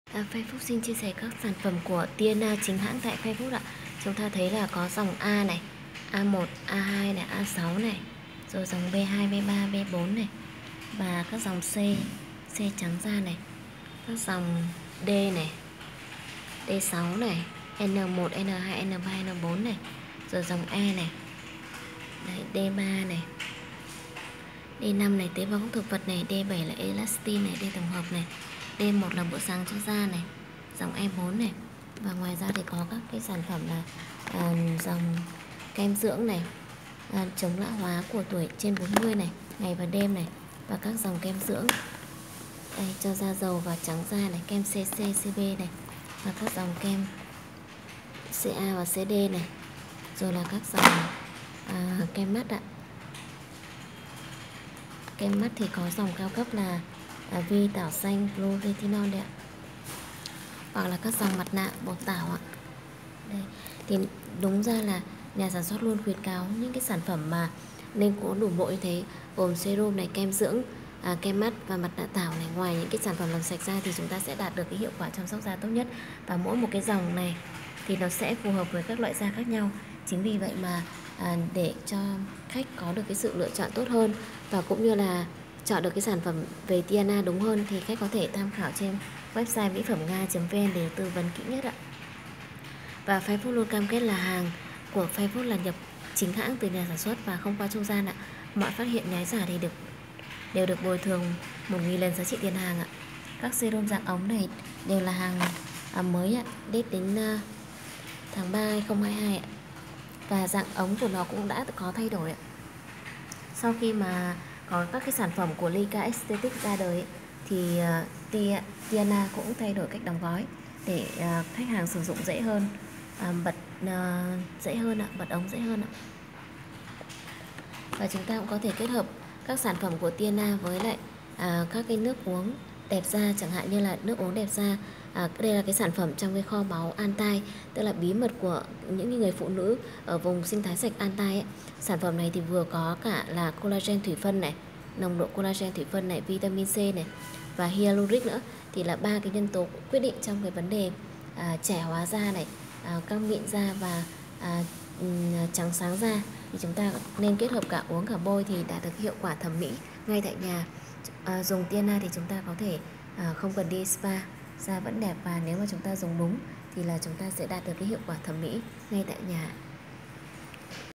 Dược mỹ phẩm Teana chuẩn chính hãng tại Fairfood, tư vấn chăm sóc da tại nhà đẹp như spa
Teana là dòng dược mỹ phẩm dạng ống mang lại hiệu quả dưỡng da an toàn, căng bóng và đẹp nhanh, nhưng hàng nhái giả từ trung quốc về quá nhiều thành ra khách nếu không tinh ý là mua ngay phải hàng kém chất lượng sử dụng hóa chất để đạt được hiệu quả.
Nhiều người nói dùng nhái và thật như nhau, có thể nói hiệu quả đạt được như nhau với 1 sản phẩm hóa chất  như một  sản phẩm tự nhiên là quá dễ ,nhưng quan trọng là bí mật trong công nghệ và thành phần tự nhiên được sử dụng để cho hiệu quả tốt.

Vậy nên nếu bạn chọn sức khỏe thì bạn chọn Mỹ_phẩm_Nga_Fairfood còn bạn quan trọng hiệu quả ngay thì đơn giản lắm, ko cần mua các sp đó làm gì ,mua hóa chất corticoid, hydroquine về tự bôi cũng đẹp mỹ mãn